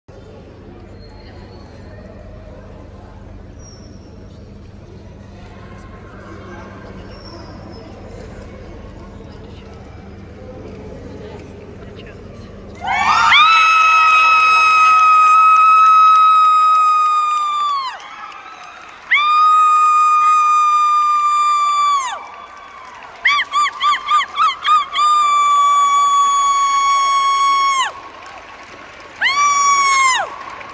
Skye Wins the 2019 Saluki National
Skye Wins the 2019 Saluki National from the 12-15 month class.